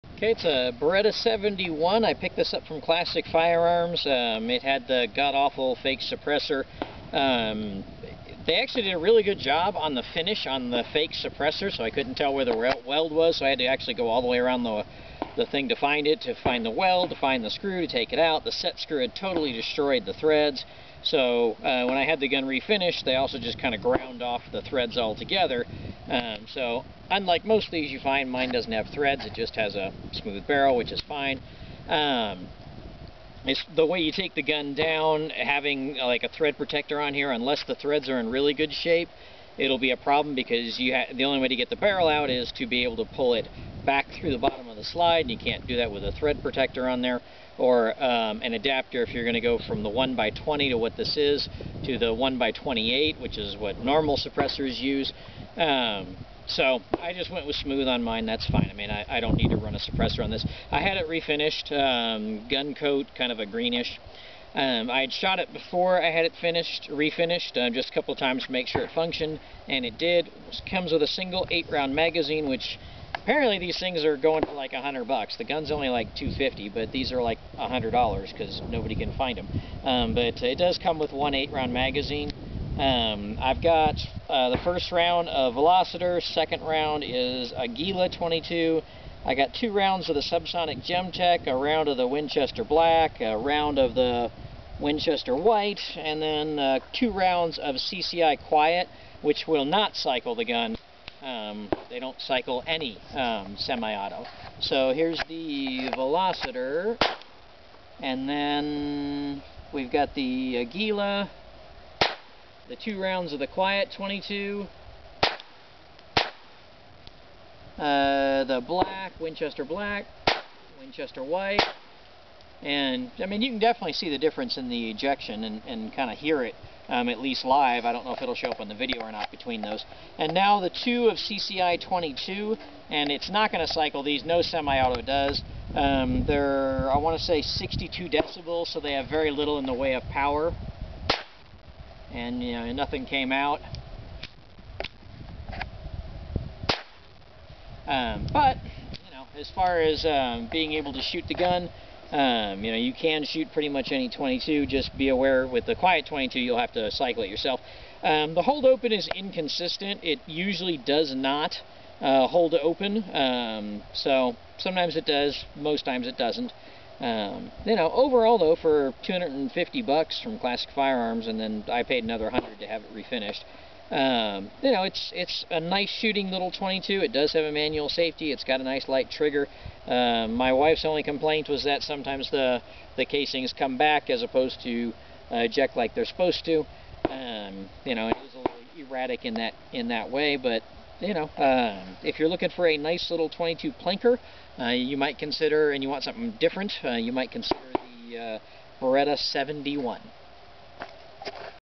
Beretta 71 Review
This is a quick review of a Beretta 71 that I picked up from Classic Firearms for about $250. I've run 8 different types of 22 through the pistol and show 6 different types in a single mag. While shooting I did mistakenly call the Gemtech sub-sonic the CCI Quiet. The Gemtech sub-sonic feeds reliably in this pistol while the very low power CCI Quiet does not. That isn't a knock on the pistol as I don't know of any semi-auto that the CCI Quiet has enough oomph to function.